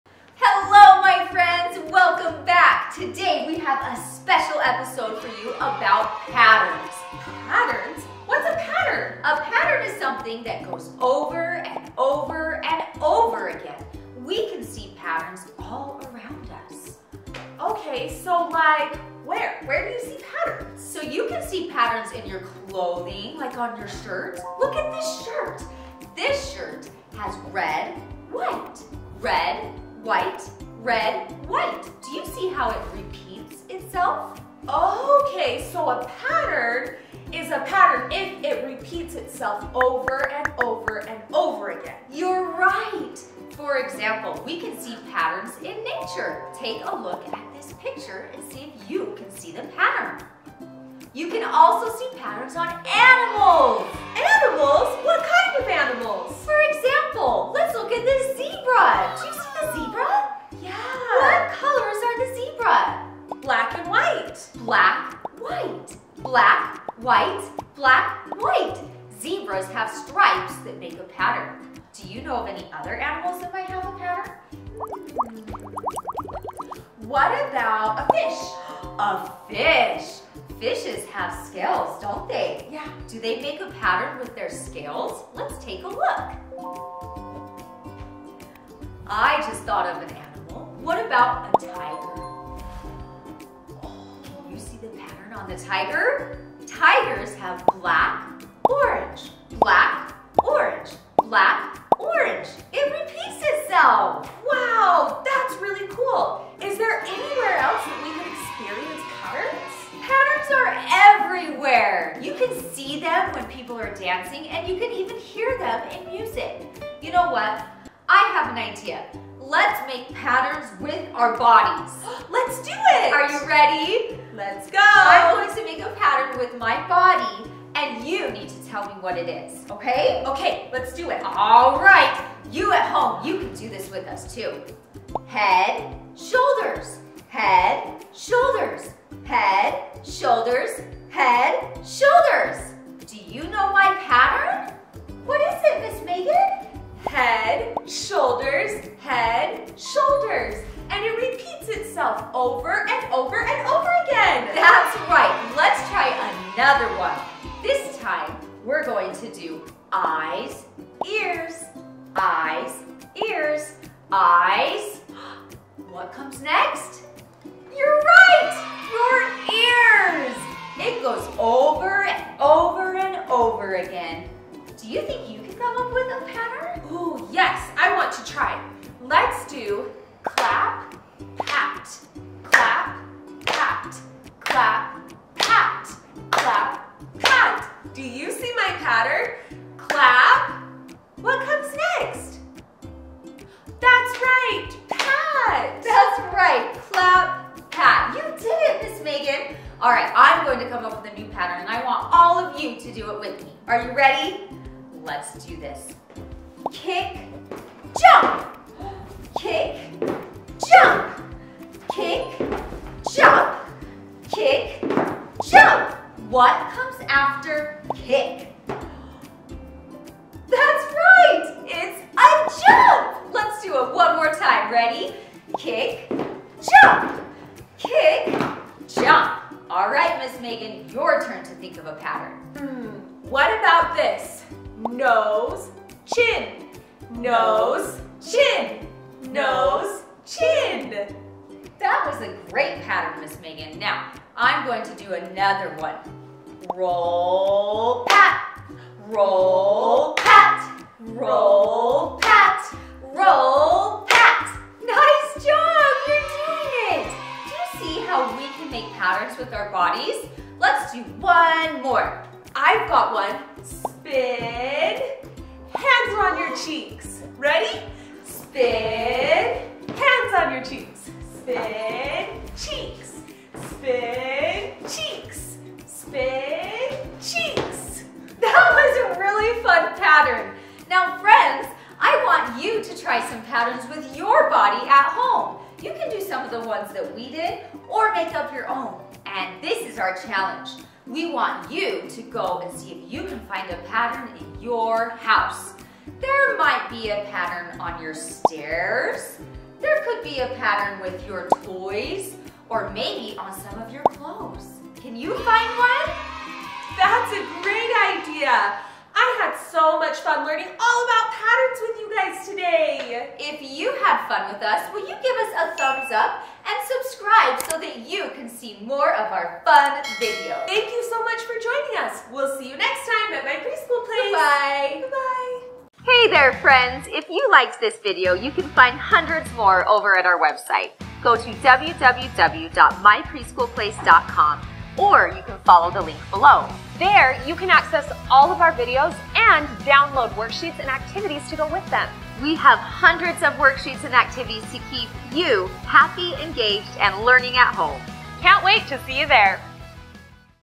Discover Patterns with The Preschool Place | Fun Learning for Kids!
🎨 Welcome to The Preschool Place, where learning is always an adventure! 🌟 In our latest video, we're diving into the captivating world of patterns. Designed especially for young learners, this engaging content will spark curiosity and creativity as kids explore the fascinating realm of shapes and colors.

🧩 Join us as we embark on a journey of pattern recognition! From simple sequences to intricate designs, children will delight in discovering and creating patterns of their own. With vibrant visuals and interactive examples, learning becomes an exciting and immersive experience.

👦👧 Our video provides the perfect opportunity for kids to develop essential cognitive skills while having fun. Whether at home or in the classroom, it's a fantastic resource for parents and educators alike to enhance early learning.